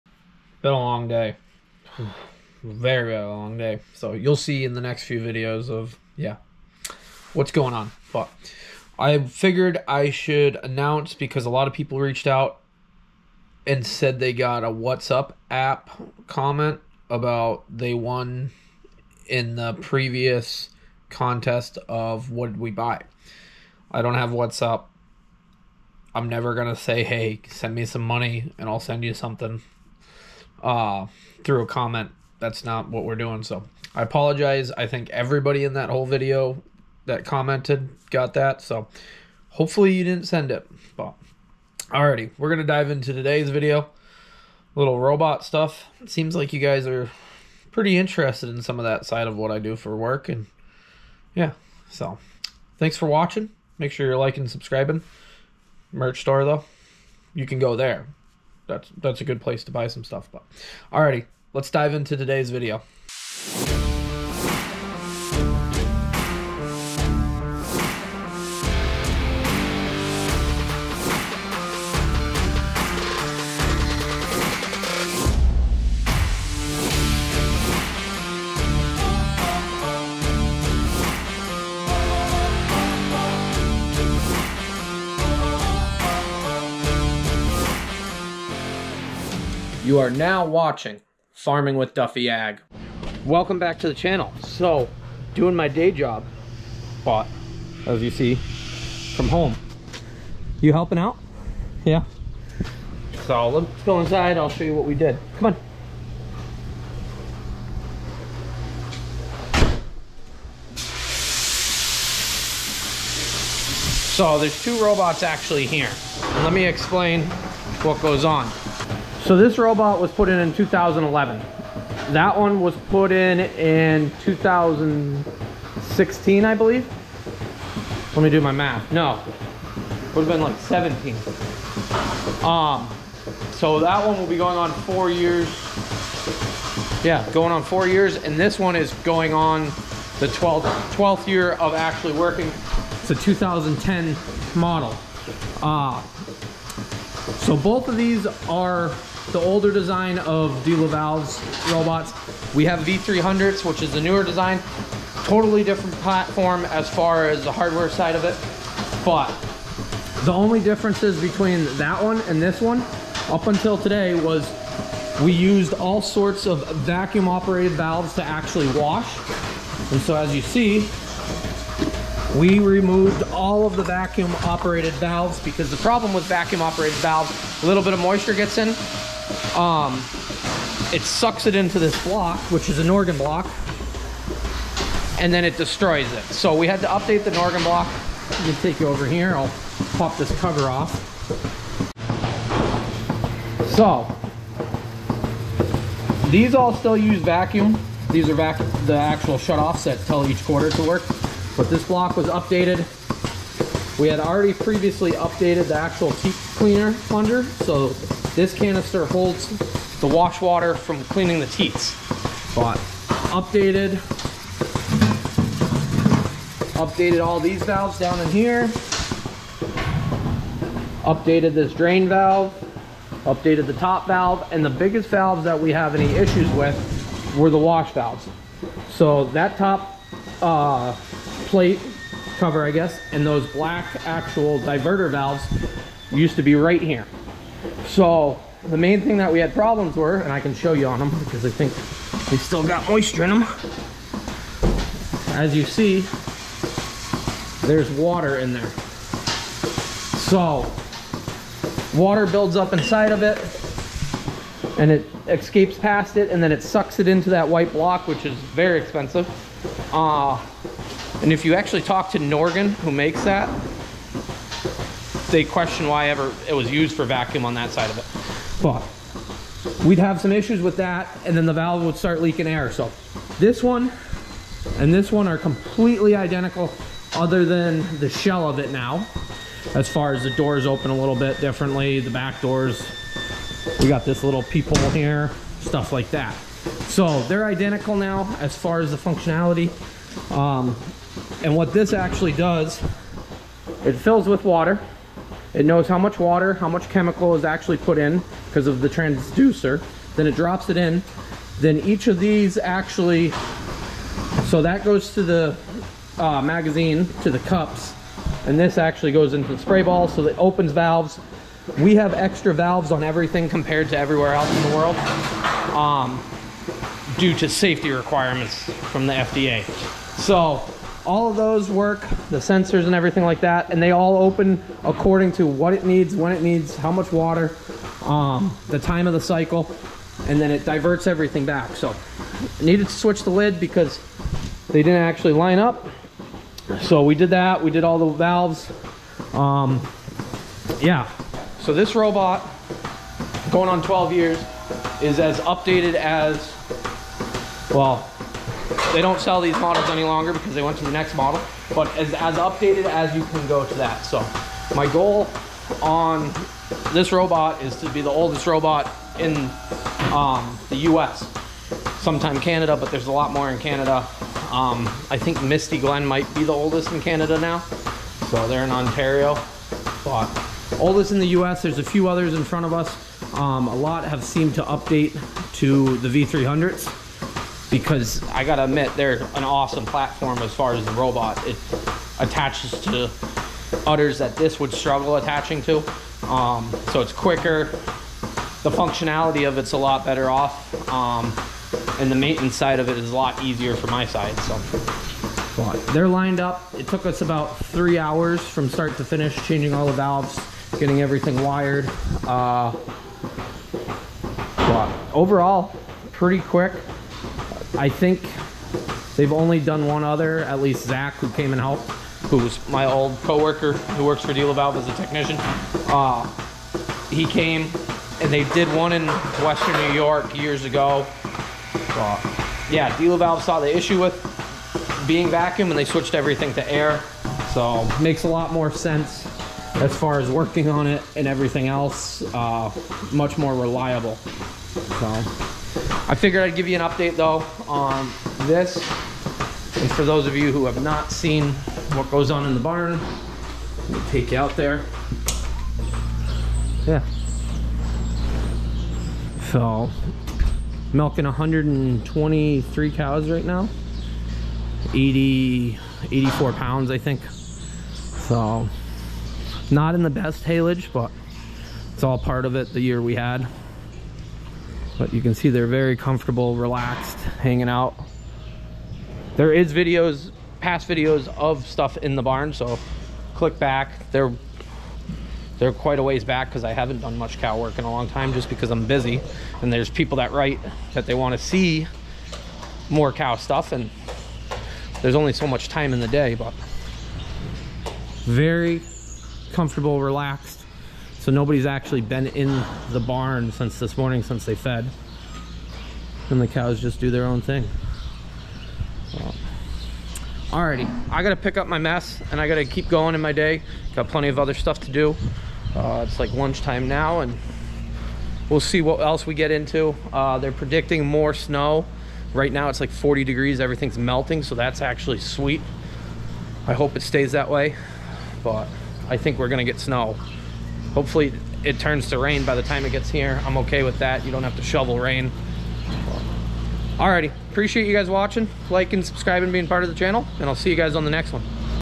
It Is Time For A Upgrade- Robotic Dairy Milking Equipment Upgrade
Join along as we update the 12 year old  Delaval milking robot from vacuum operated valves to air operated valves.